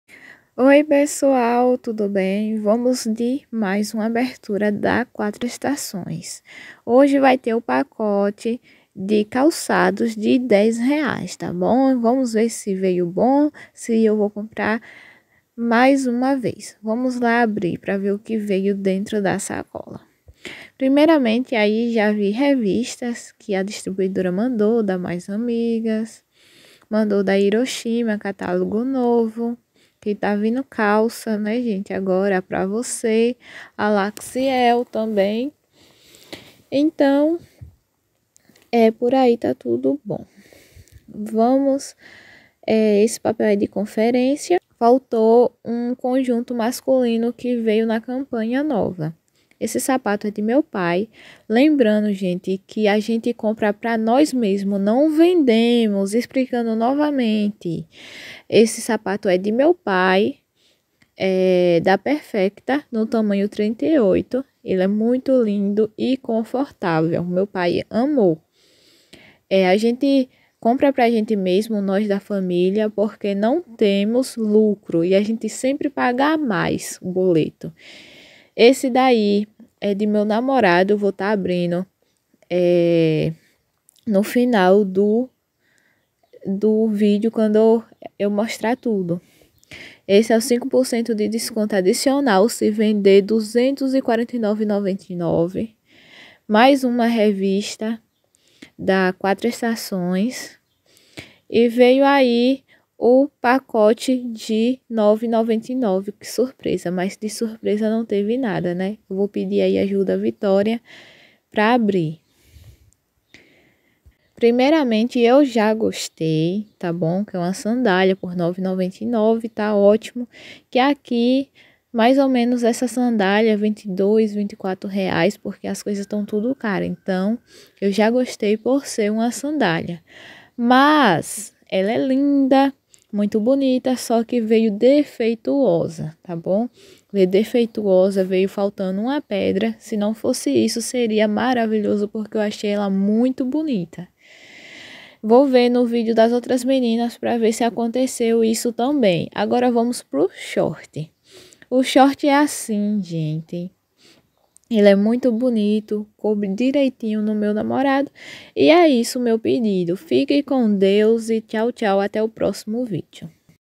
ABERTURA DE SACOLA DA QUATRO ESTAÇÕES | com pacote de calçados
o vídeo de hoje é sobre abertura de sacola da Quatro Estações.
foram pouco itens, mas nela vai estar o novo pacote surpresa de r$ 9,99 de calçados. então espero que vocês gostem do vídeo...
um beijo! 🤭😘